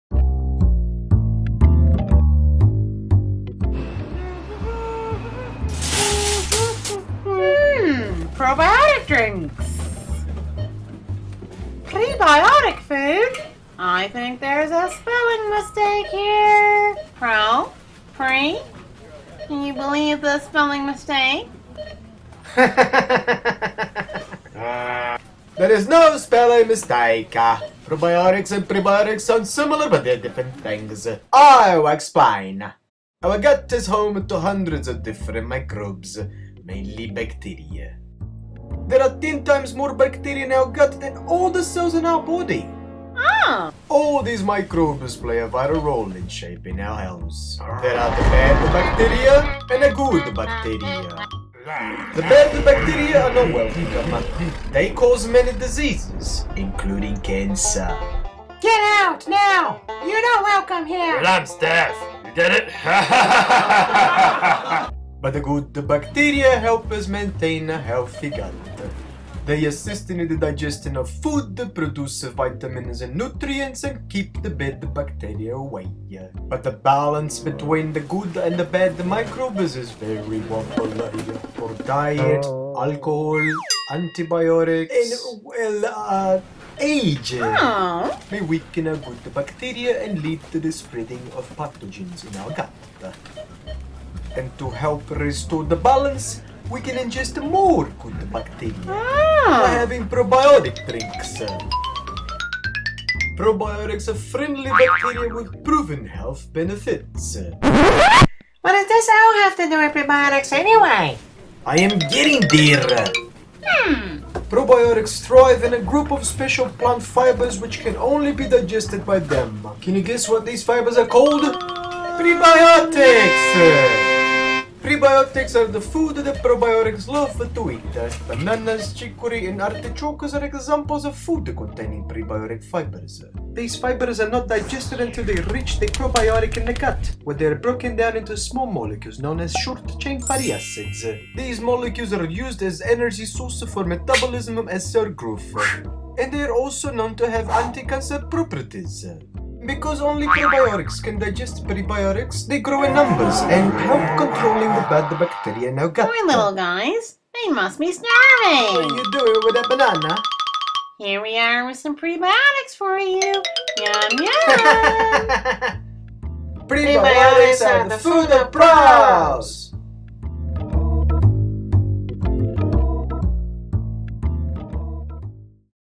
Prebiotics, the food of Pros
Prebiotics, the food of Pros   was made with the purpose of educate people on the diference between probiotics and prebiotics.    It unveils that our gut is home to hundreds of different microbes, including good and bad bacteria. These bacteria play a vital role in keeping us healthy.    As an effort to guarantee our gut is colonized by the right type of microbes (good bacteria), one could ingest bacteria with proven healthy benefits known as Probiotics.    These healthy bacteria are commonly added to yoghurt and other drinks.  To boost the healthy benefits conferred by Probiotics, one could consume foods with high contents of special fibers, which can only be digested by the Probiotics.    These fibers, known as Prebiotics, are found in foods, such as bananas, chicory, garlic and artichoke.     The products of Prebiotics digestion, short chain fatty acids (SCFA), are then used in functions, such as metabolism and cell growth. SCFAs have also been shown to have anti-cancer properties.    I hope you enjoy this short animation. It was my first one